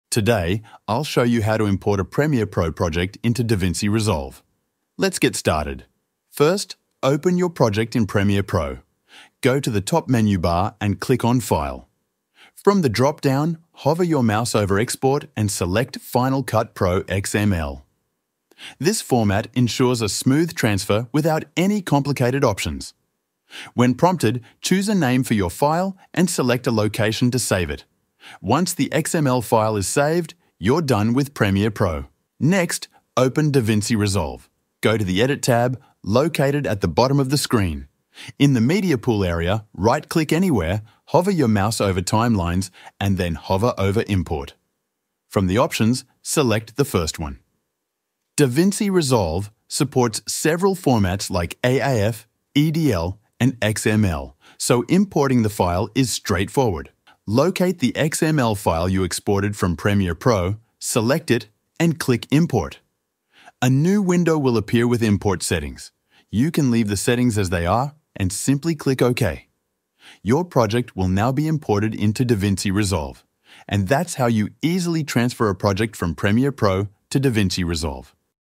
Import Premiere Pro Project into DaVinci Resolve (Tutorial)
In this short tutorial I show you how you can import Premiere Pro Project into DaVinci Resolve step-by-step.